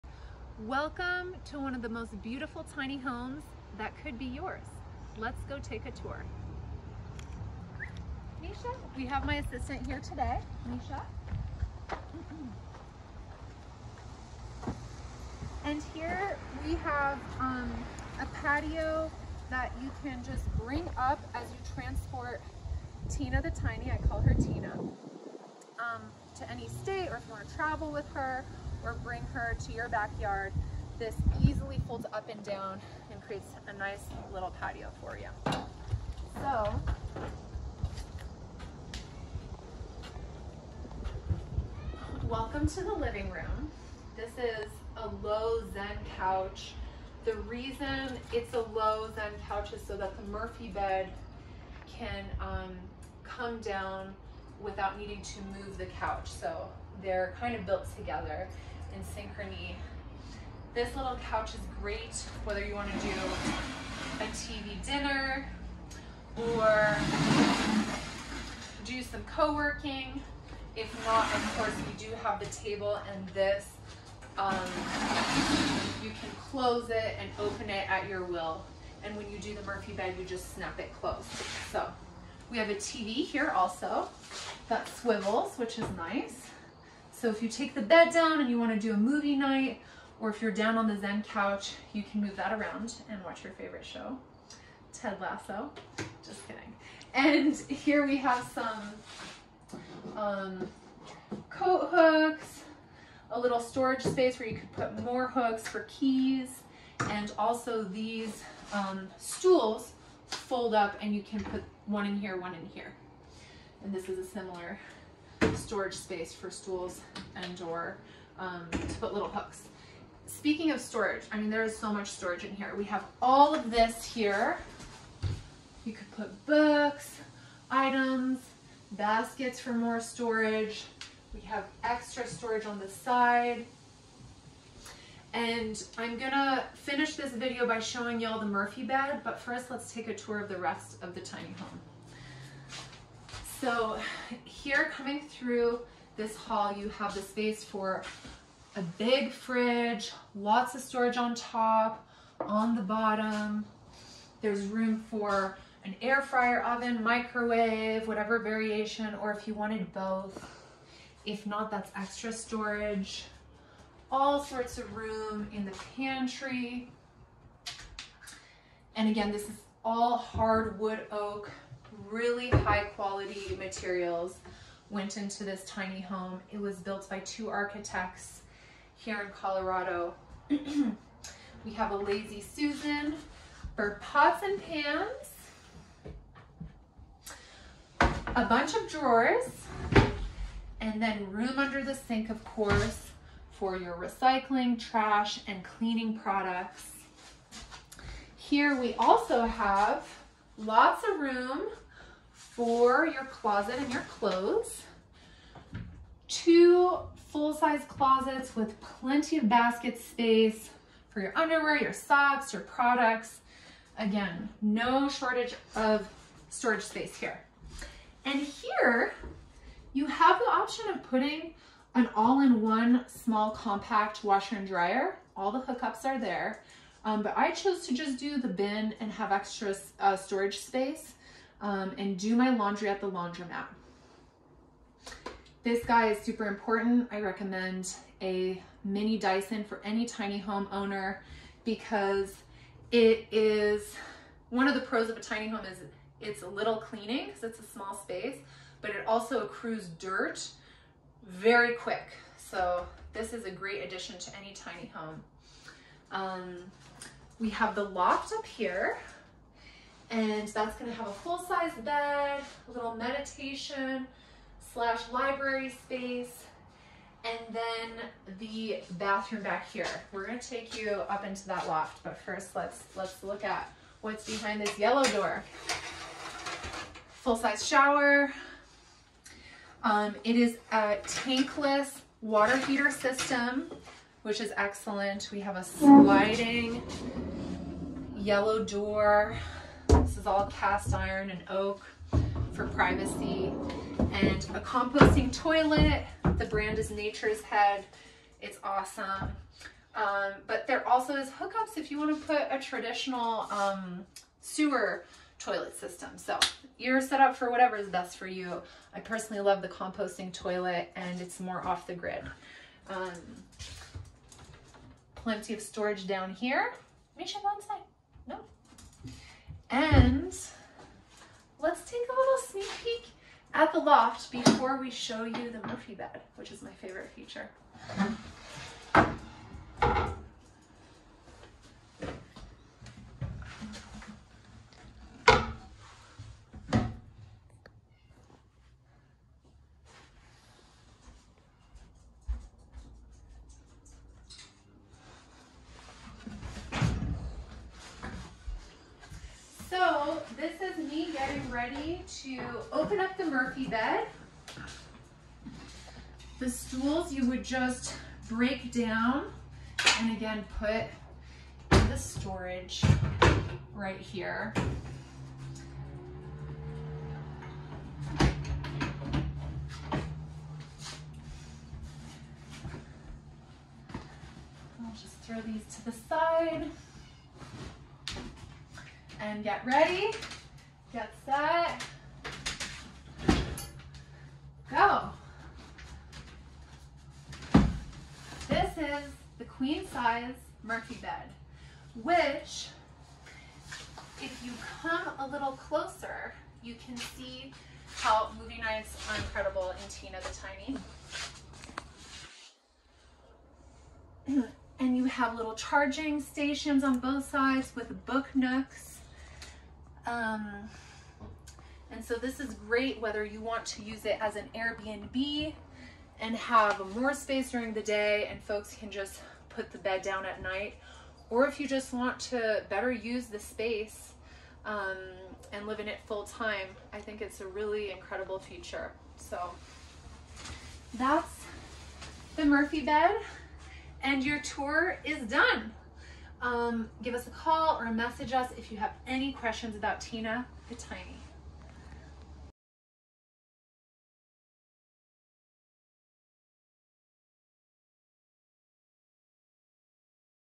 We Think This Might Be The Tiny House For You
"Check out this one-of-a-kind modern Tiny Home! This home was professionally built by a licensed custom home Design-Build contractor. I bought it as an investment and it will be permanently stationed in my big garden outback."